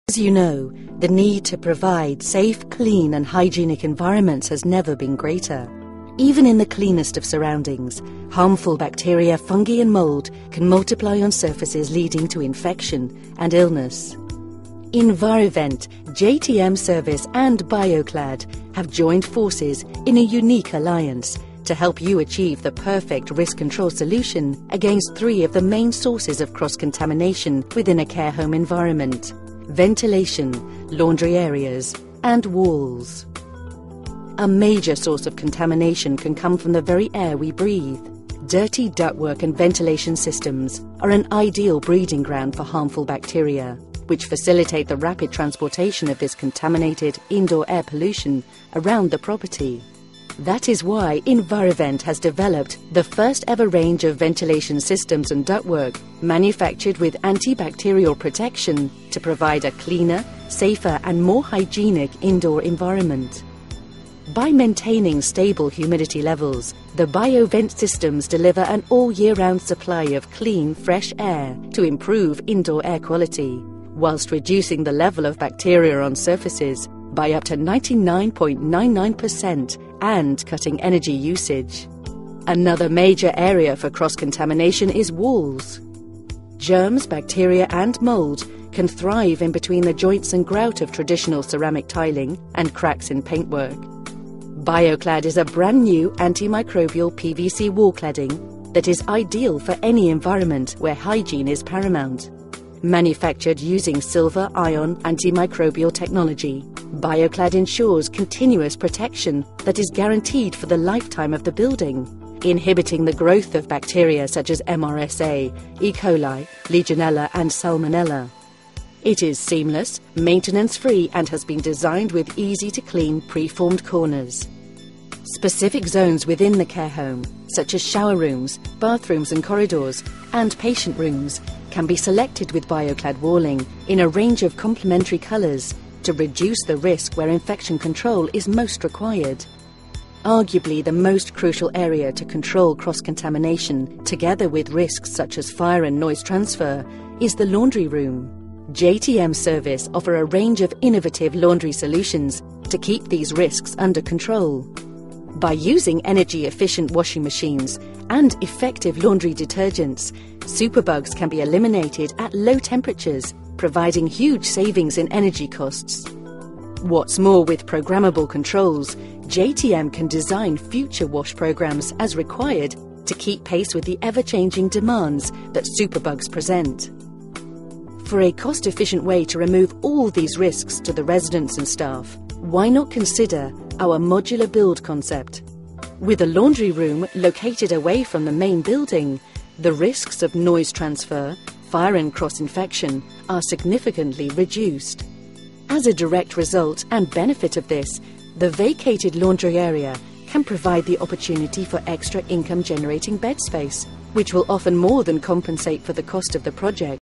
Removing risk from care homes
JTM have partnered with two other leading companies in the care home sector to provide innovative solution in taking the risk of infection out of the care home environment.

JTM Service specialise in laundry solutions for the care home sector.